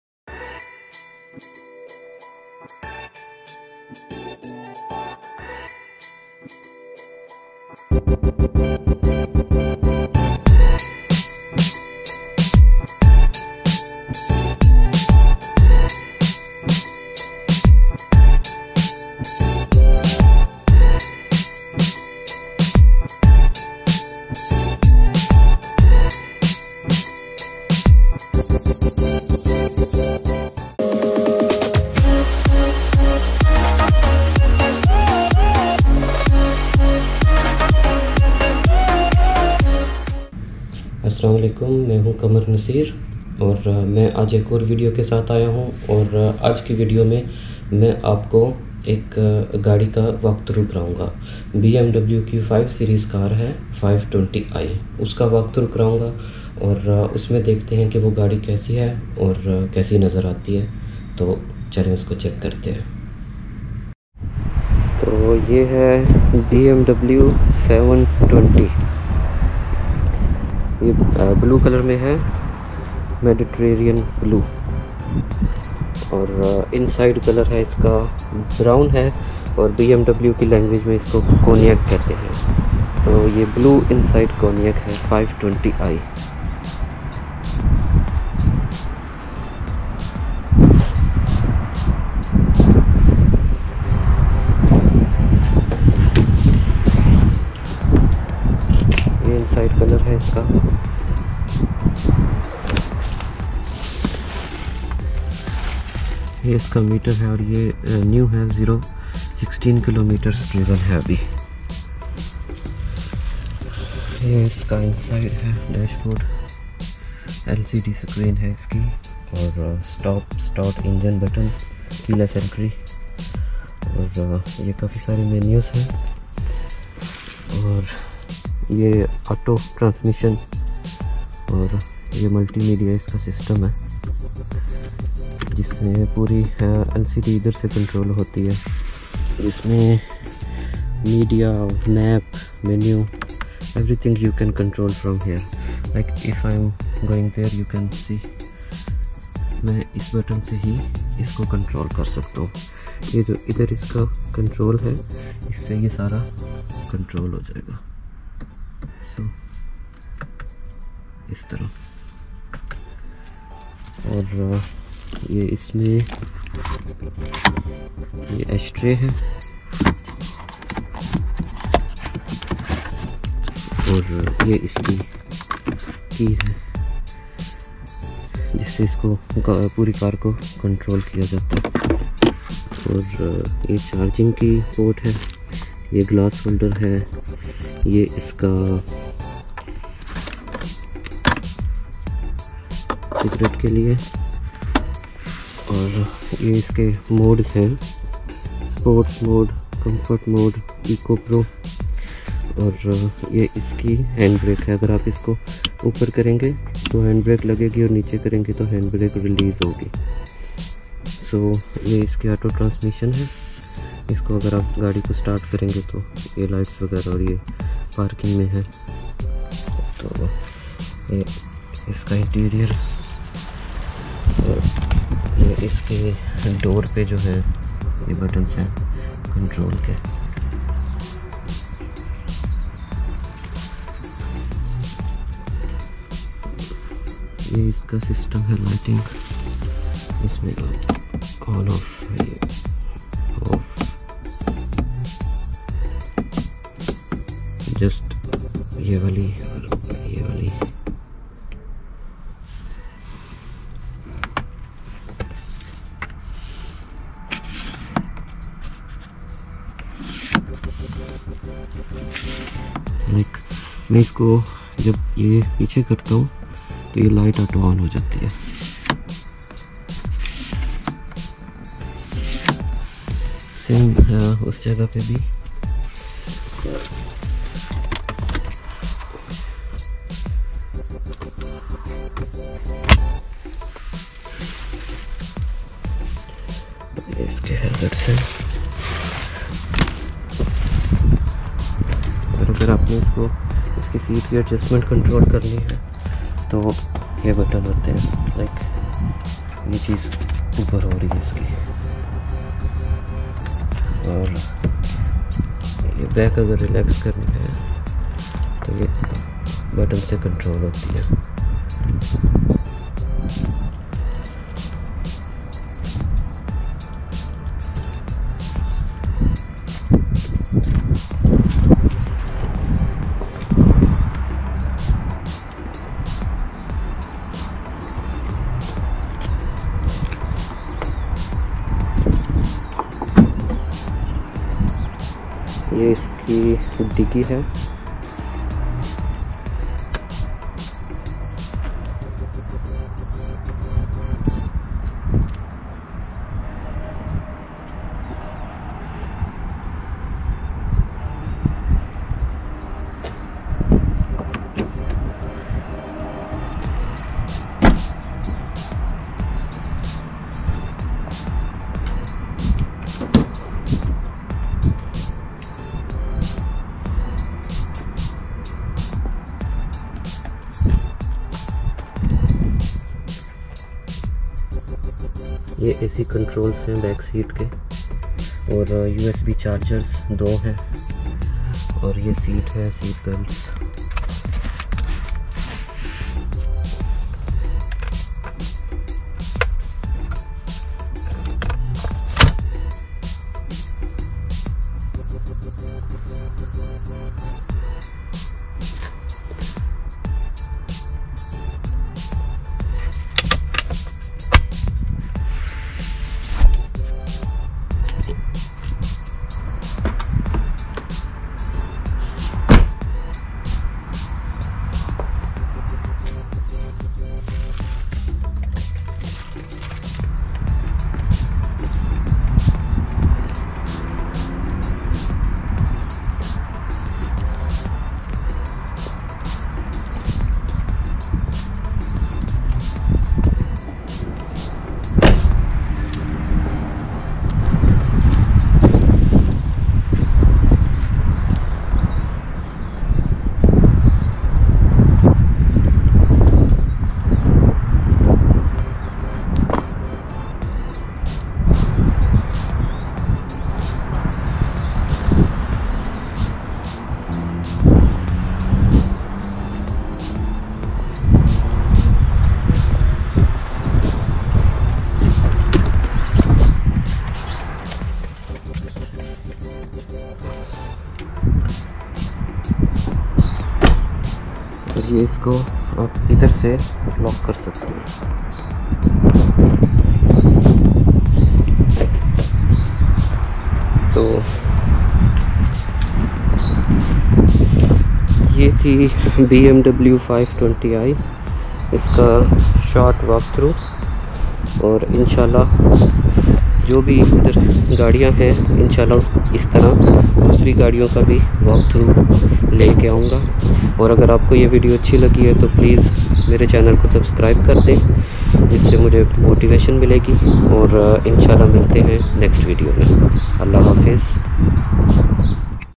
BMW 520i 2020 Walk Around | Car Review | Qamar Naseer
In this video i am giving walk around and short review of New BMW 520 i 2020 Model